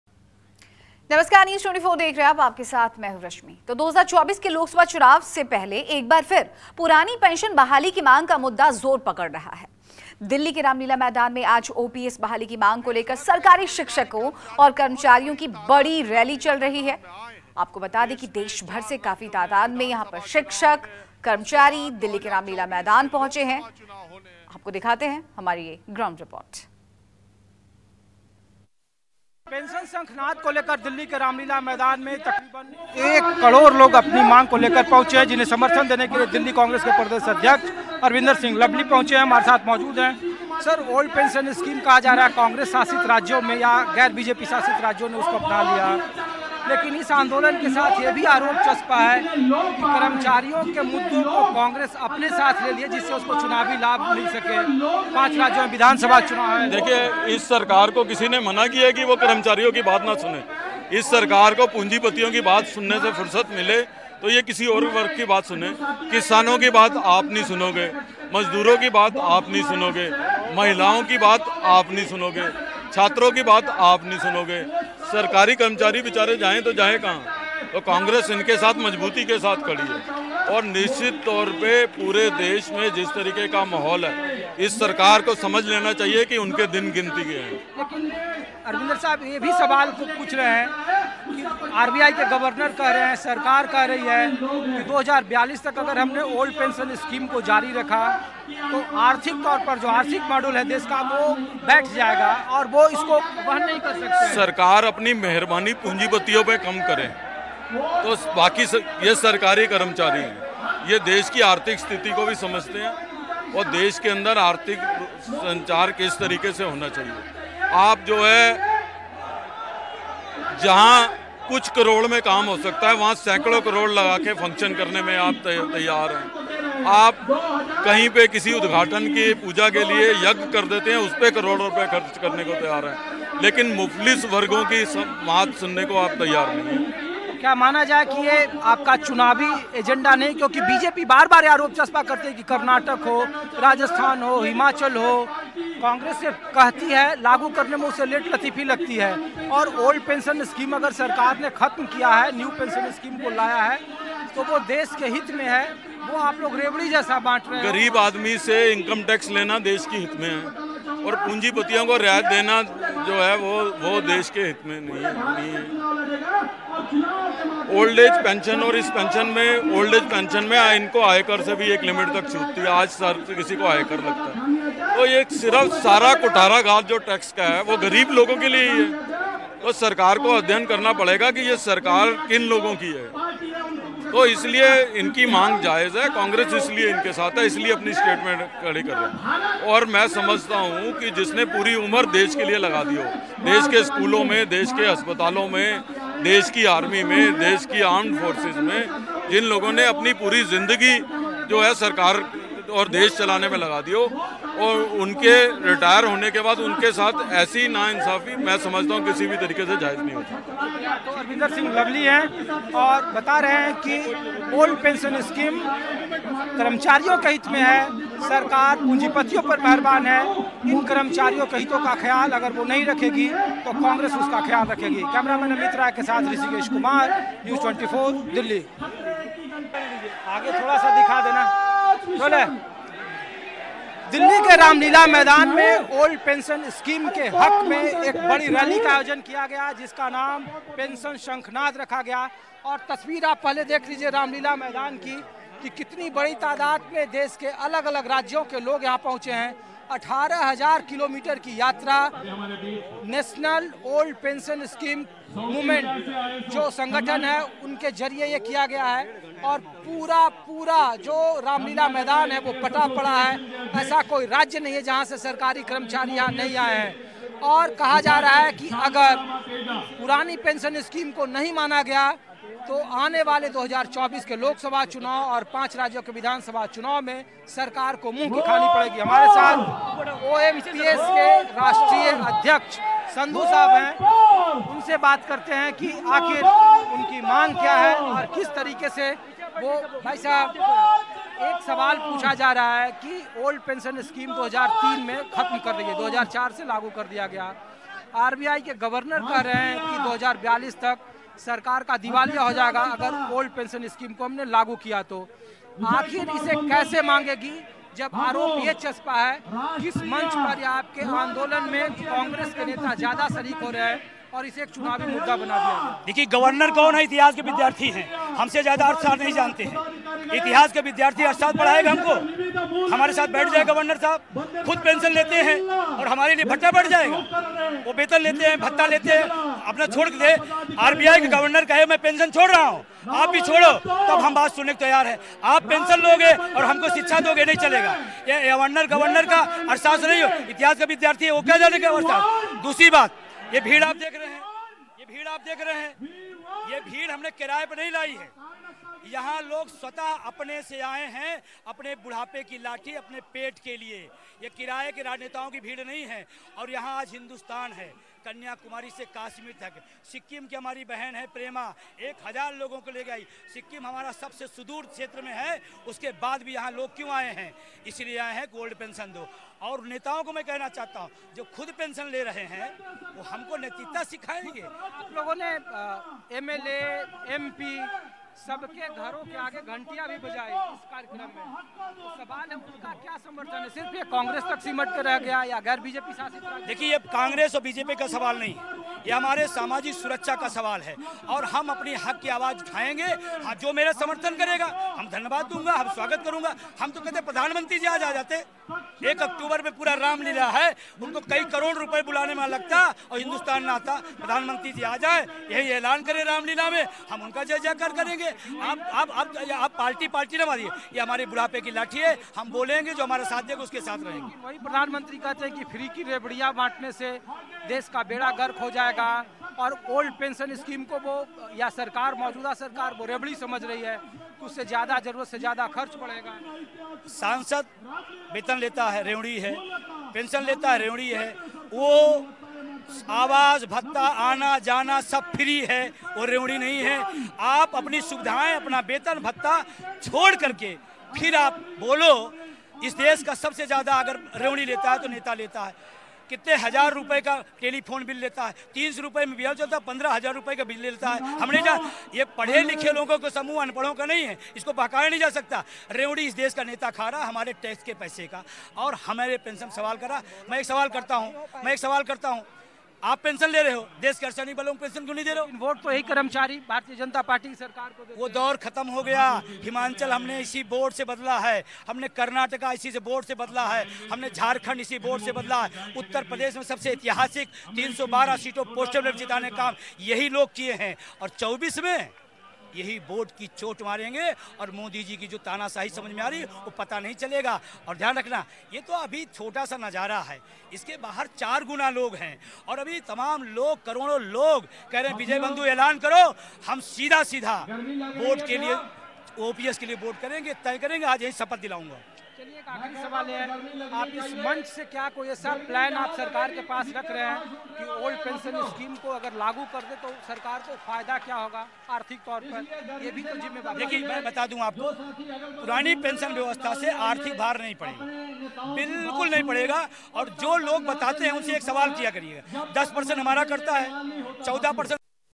रामलीला मैदान में जुटे हजारों कर्मचारी…PM Modi से OPS और निजीकरण खत्म करने की मांग | ग्राउंड रिपोर्ट
रामलीला मैदान में जुटे हराजों कर्मचारी…PM Modi से OPS और निजीकरण खत्म करने की मांग | ग्राउंड रिपोर्ट